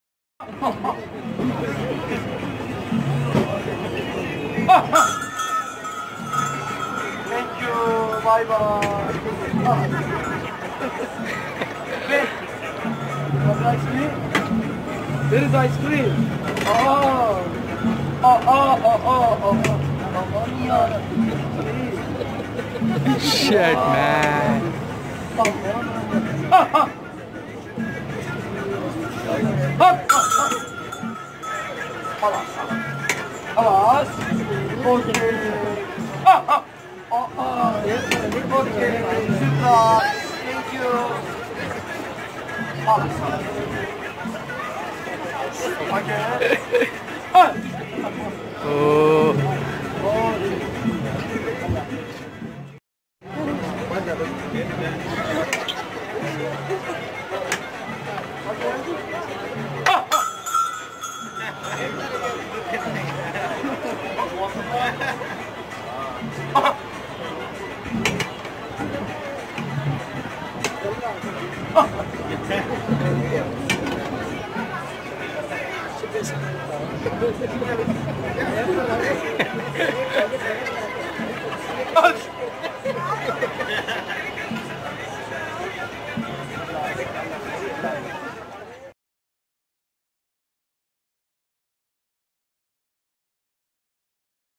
Ice cream sale funny way irrani man in souq waqif
This video from Qatar irrani market that guy saling ice cream with entertain people. To this is awesome